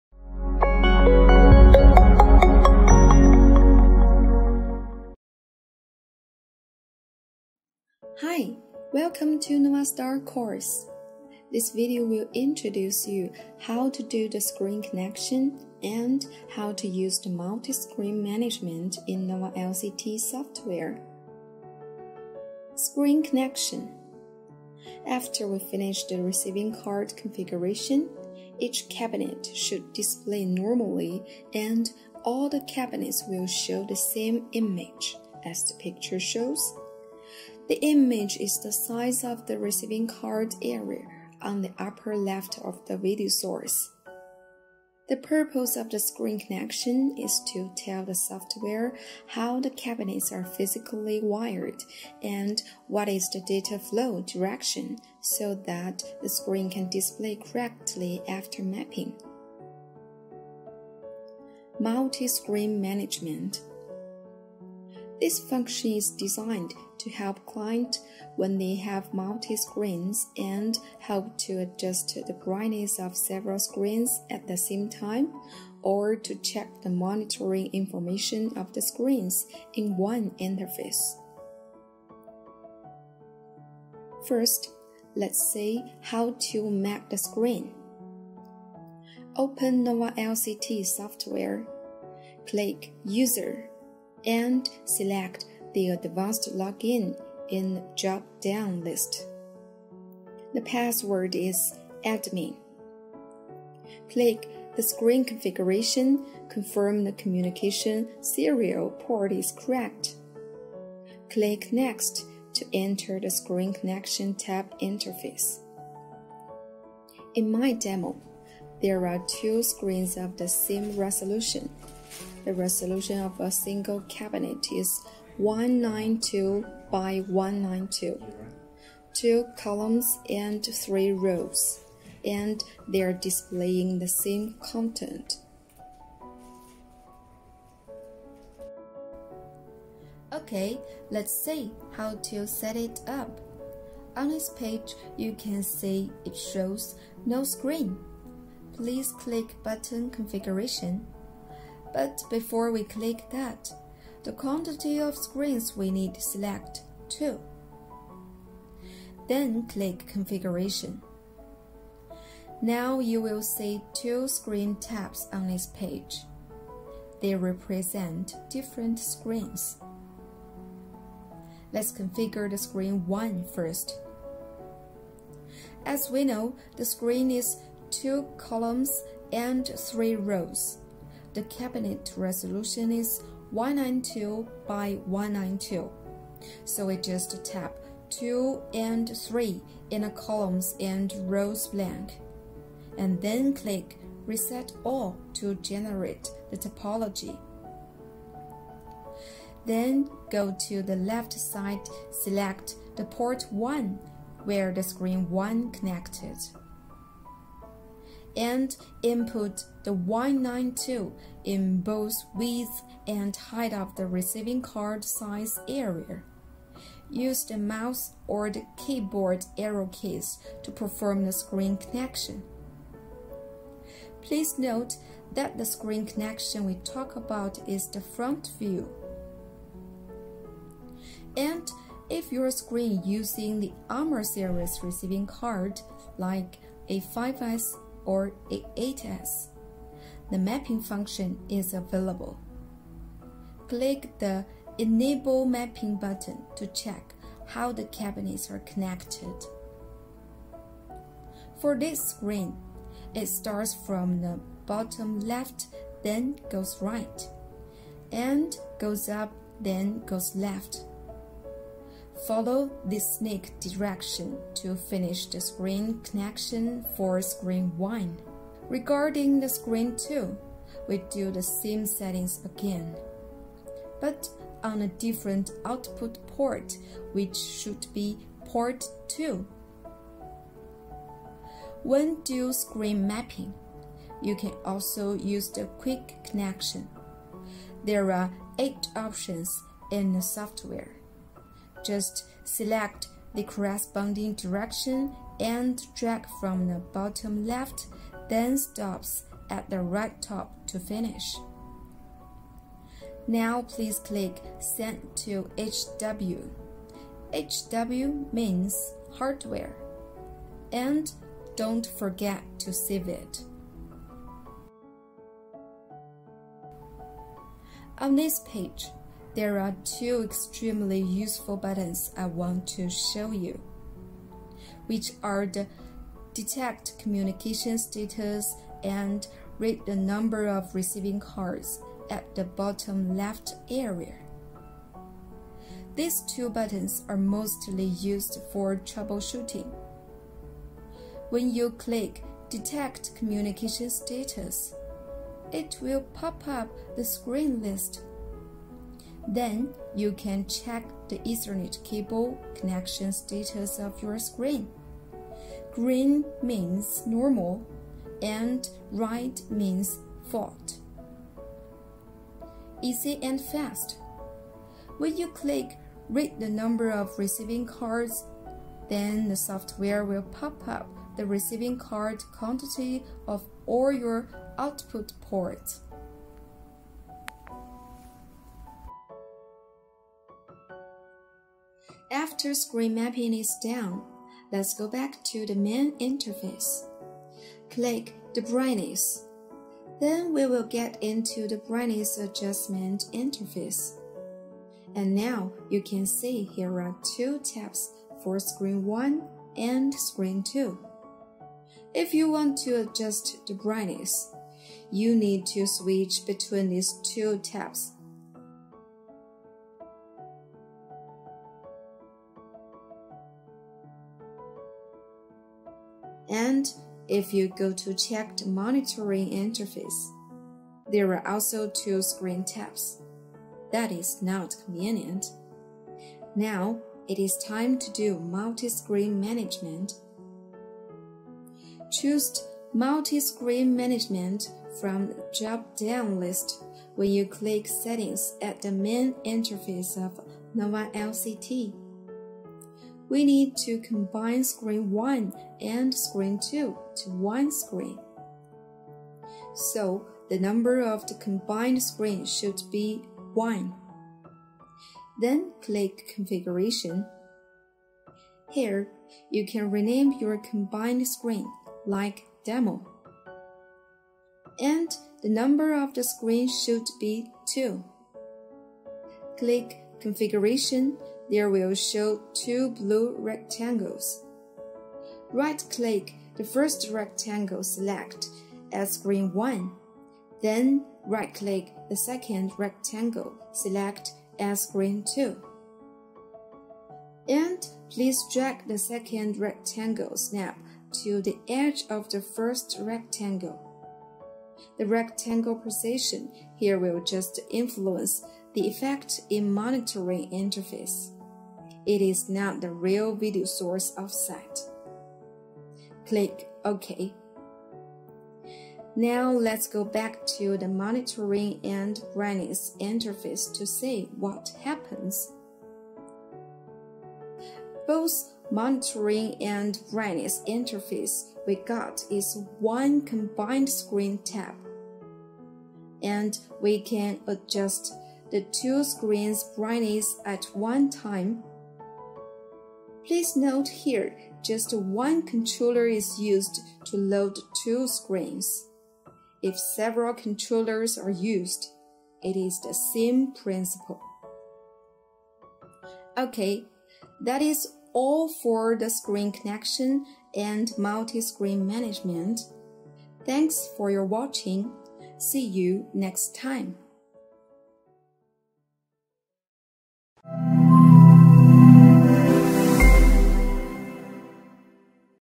04 Подключение экрана и управление несколькими экранами
Посмотрите это видео, в котором вы узнаете, как настроить программное обеспечение после физического подключения кабинетов для корректного изображения после мэппинга.
Кроме этого, видео также знакомит вас с тем, как регулировать яркость или проверять информацию о мониторинге, если у вас несколько экранов.